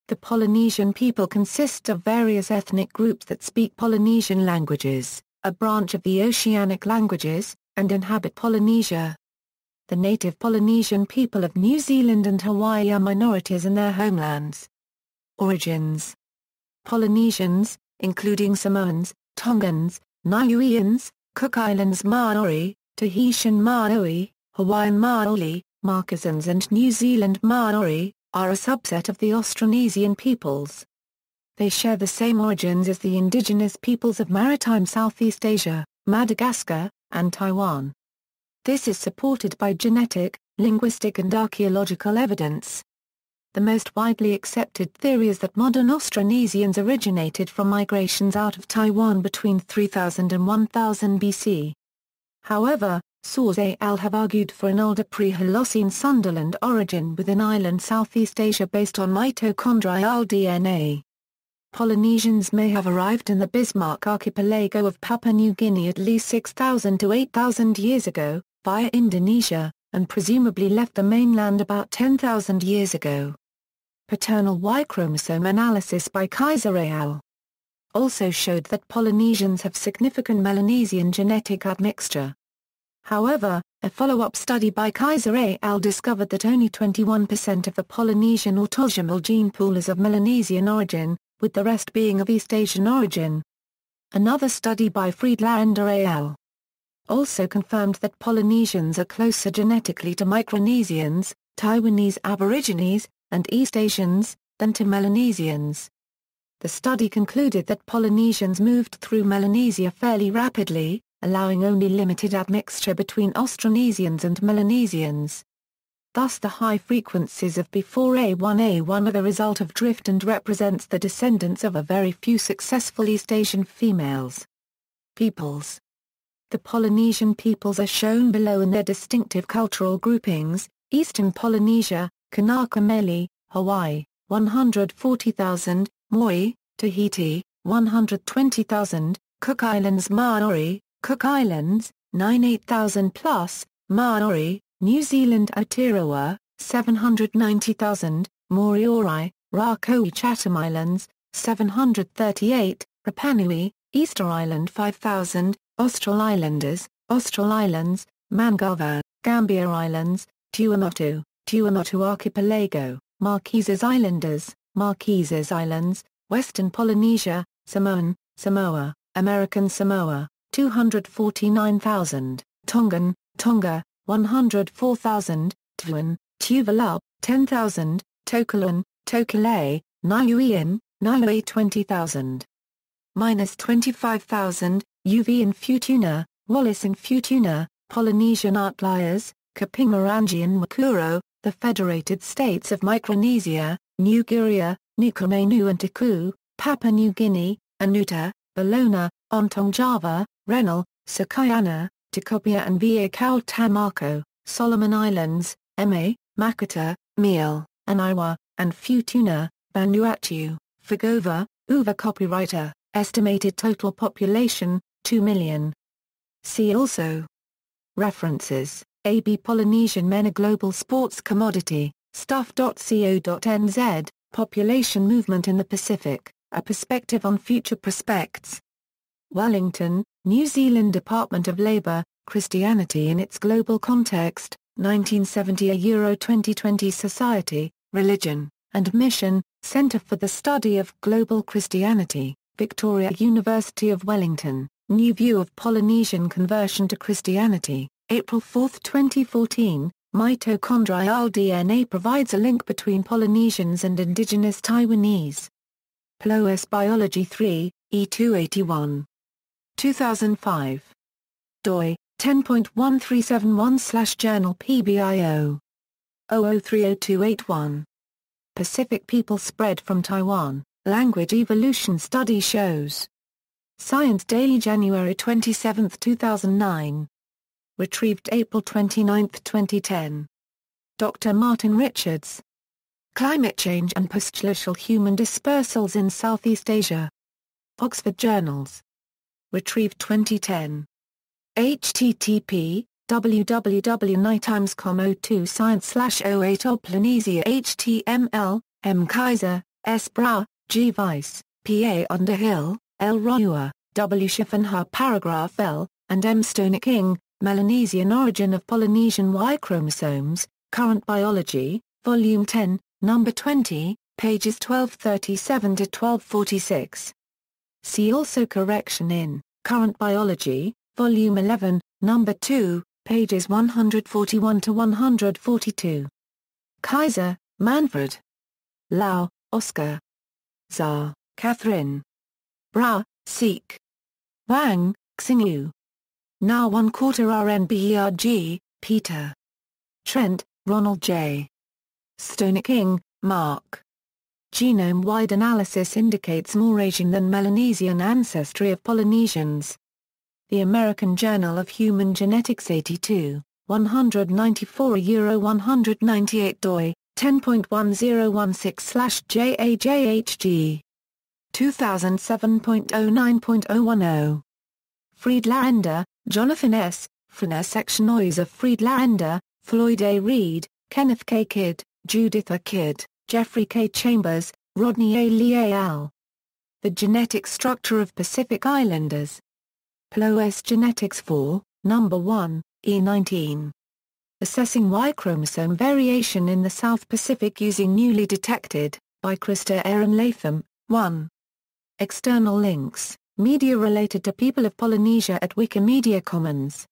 Polynesians
The Polynesian people consists of various ethnic groups that speak Polynesian languages, a branch of the Oceanic languages, and inhabit Polynesia. The native Polynesian people of New Zealand and Hawaii are minorities in their homelands.